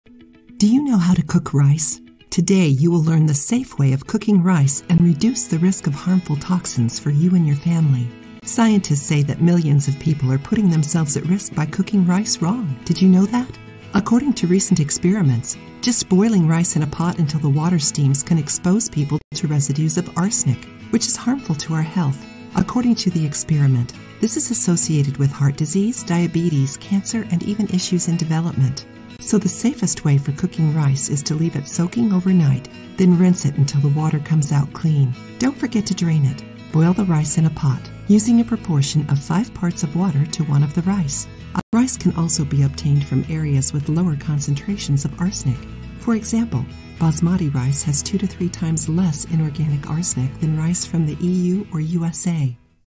You’re Cooking Rice Wrong and It’s Dangerous
How to Cook Rice to Remove the Most Arsenic?

Do you know how to cook rice?

Today you will learn the safe way of cooking rice and reduce the risk of harmful toxins for you and your family.

Scientists say that millions of people are putting themselves at risk by cooking rice wrong. Did you know that?

According to recent experiments, just boiling rice in a pot until the water steams can expose people to residues of arsenic, which is harmful to our health.

According to the researcher, rice is the only grain cultivated in flooding conditions.

The flooding releases inorganic arsenic, usually contained in soil minerals, making it available to the plants.

The European Food Safety Authority says that rice has 10 times more inorganic arsenic than other foods.

This makes people who eat a lot of rice more exposed to alarming concentrations of this poison.

There are many practical solutions to remove inorganic arsenic from rice, from crop management, selection, and improvement of crops, to the cooking process.

Now that you know how to cook your rice correctly, try it and see the difference it does in your health!